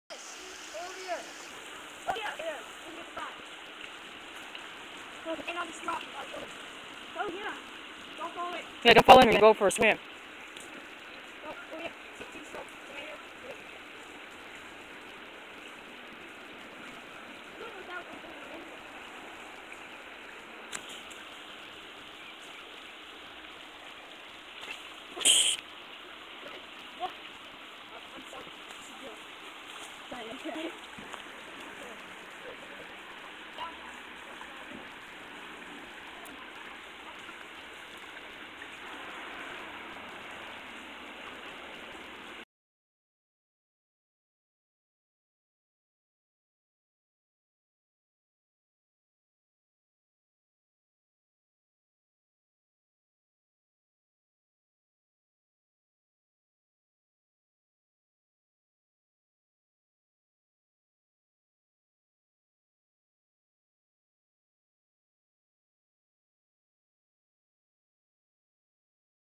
Rodney and Nicholas at Creek
Rodney and Nicholas at Woods Creek Park.